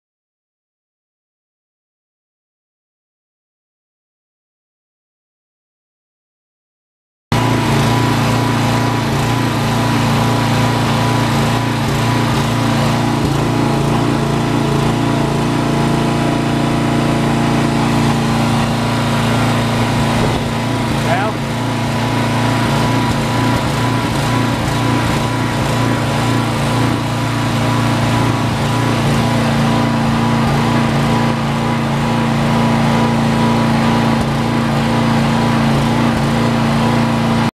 2001 Witzco Challenger RG-50 lowboy trailer for sale | sold at auction July 16, 2015
2001 Witzco Challenger RG-50 lowboy trailer
48&#039;L x 102"W
22&#039;5" well
Deck extenders
Wood floor
D-ring tie downs
Briggs & Stratton 11 HP gas engine
Spring suspension
Air brakes
11R22.5 tires
50 ton capacity
1W8A11E361S000270

It was located in Webb City, MO, and we sold it to the highest bidder for $17,050.